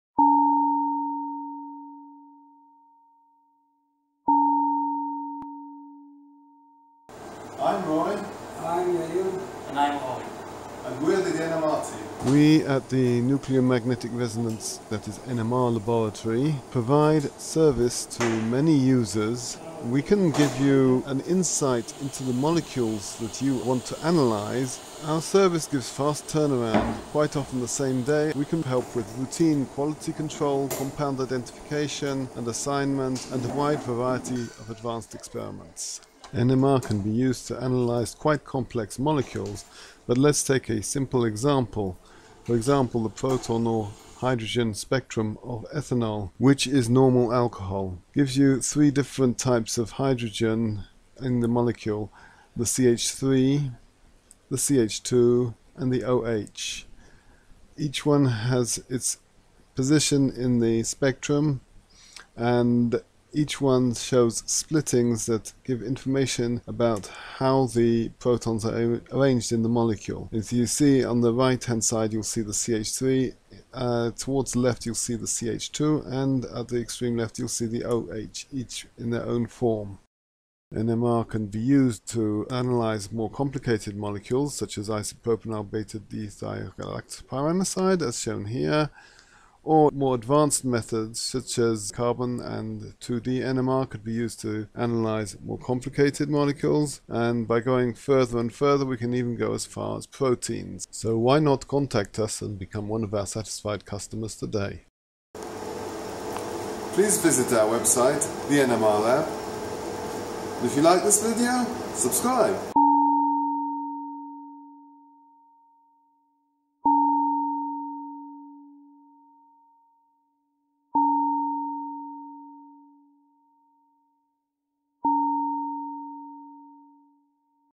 Introduction to the NMR lab
NMR service from the NMR team.
A basic introduction to our services.